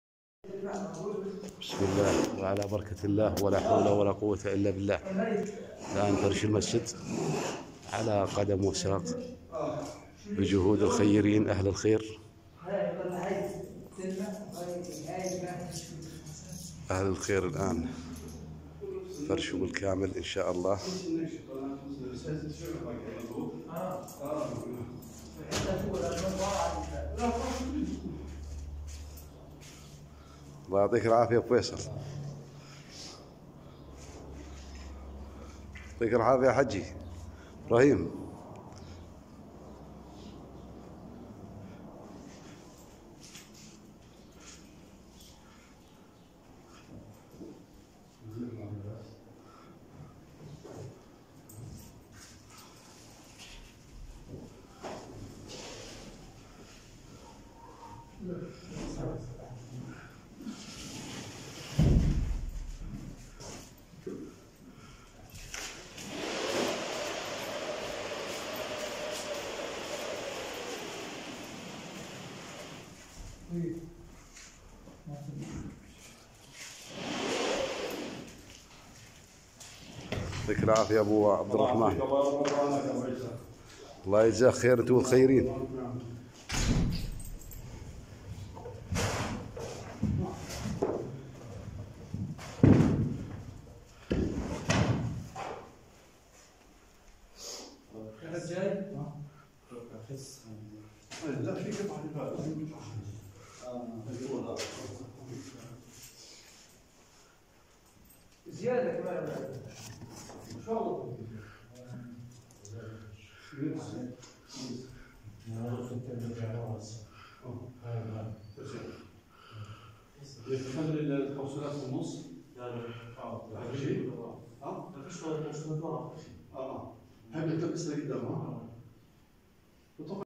إنهاء صيانة مسجد بلال بن رباح
تم إنهاء صيانة مسجد بلال بن رباح في منطقة ماركا خالد بن الوليد على نفقة اهل الخير